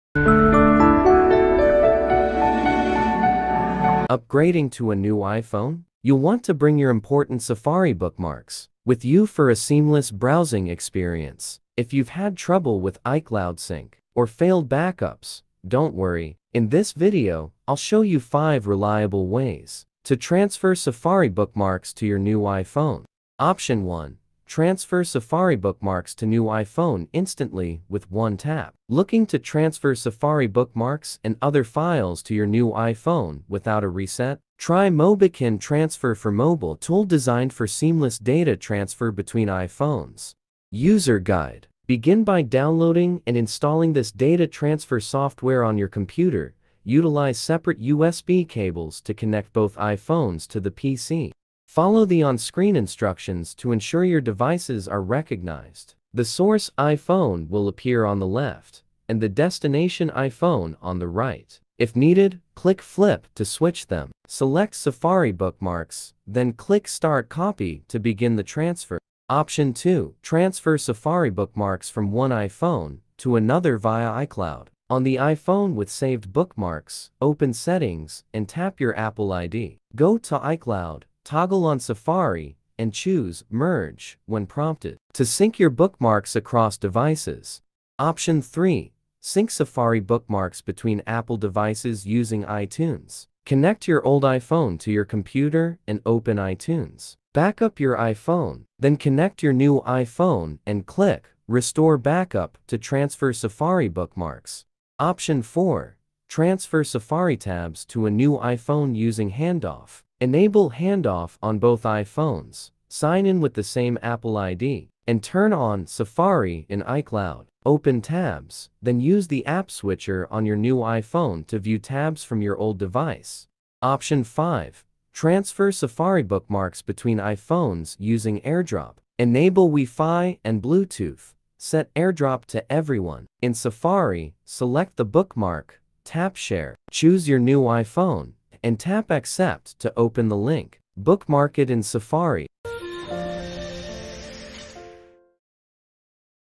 How to Transfer Safari Bookmarks to New iPhone 16/15/14? 5 Tactics
How do I sync Safari tabs between Apple devices? Follow this easy guide to learn how to transfer Safari Bookmarks to new iPhone 16/15/14/13/12/11/X/8 differently, including the best one.

00:00 Start
00:23 Option 1: Transfer Safari Bookmarks to New iPhone Instantly with One Tap
01:09 Option 2: Transfer Safari Bookmarks from One iPhone to Another via iCloud
01:27 Option 3: Sync Safari Bookmarks between Apple Devices Using iTunes
01:44 Option 4: Transfer Safari Tabs to a New iPhone Using Handoff
02:03 Option 5: Transfer Safari Bookmarks between iPhones Using AirDrop